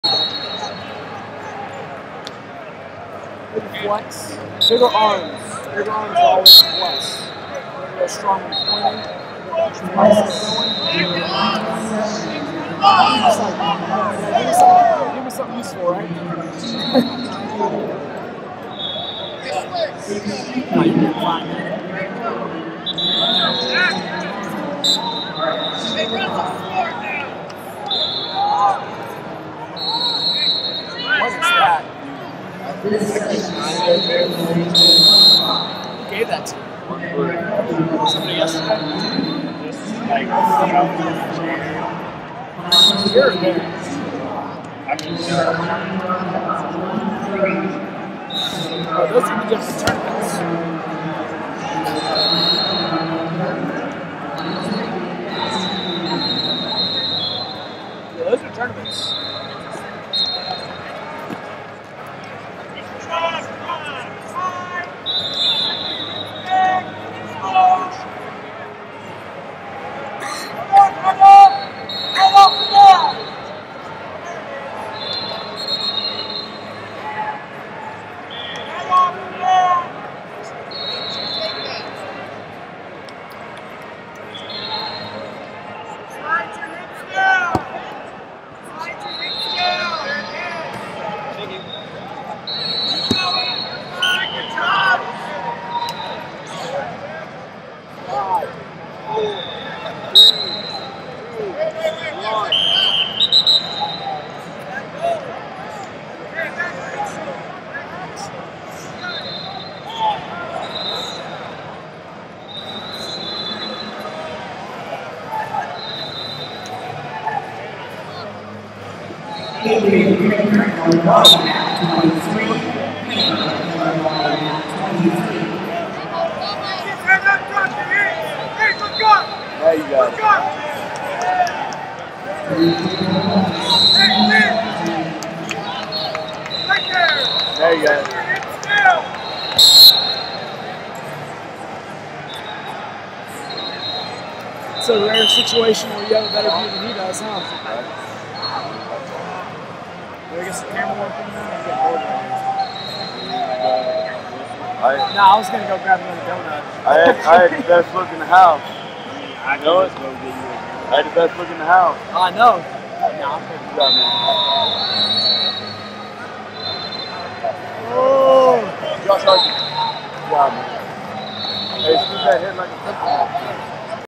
182 Lbs Rnd Of 64 Kalyn Jahn Wisconsin Vs Josh Harkey California
2019 USMC USAW Junior And Cadet National Championships - 182 Lbs Rnd Of 64 Kalyn Jahn Wisconsin Vs Josh Harkey California